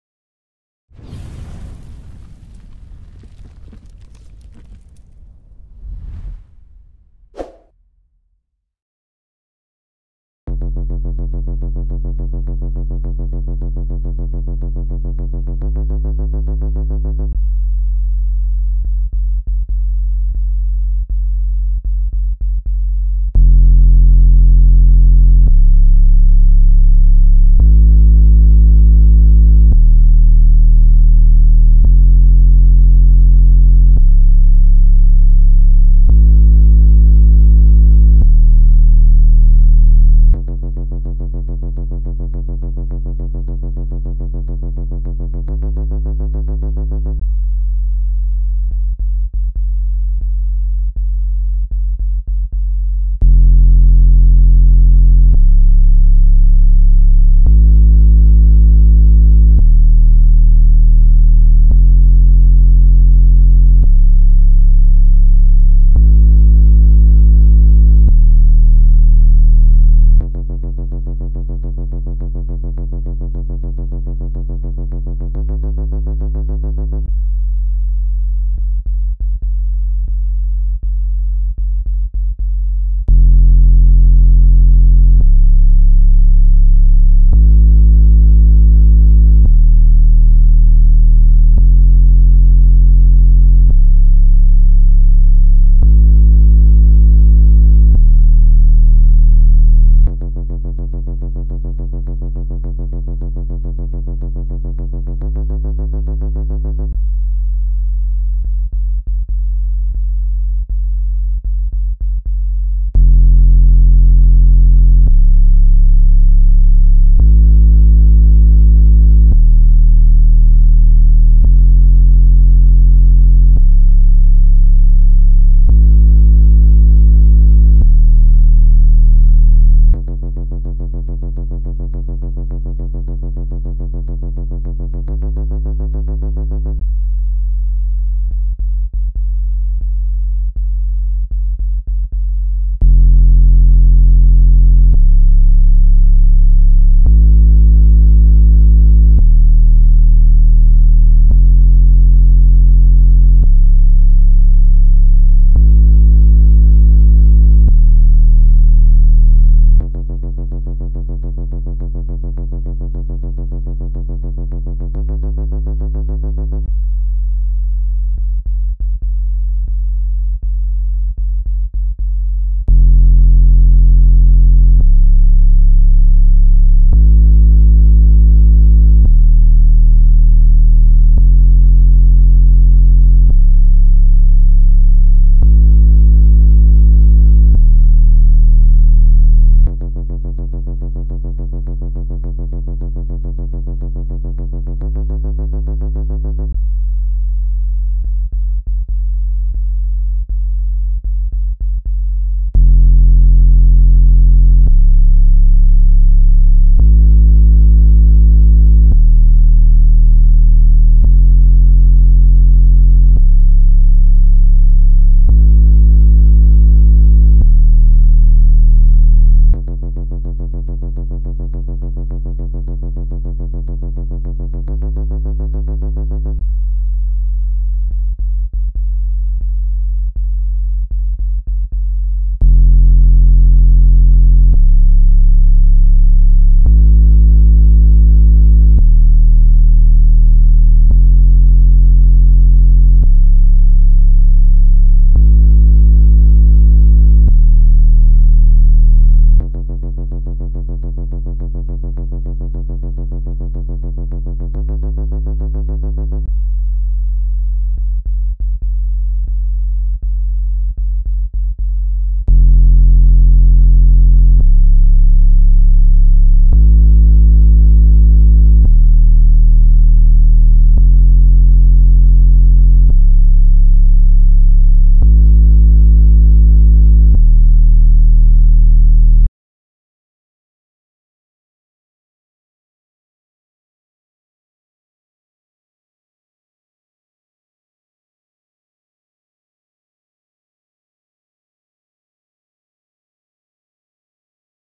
Sound To Remove Spilled Water On Laptop Speaker (GUARANTEED)
Sound to remove spilled water from laptop speakers, This sound will fix completely remove and eject water from your laptop speakers.This is the best way to get spilled water out of your laptop speaker.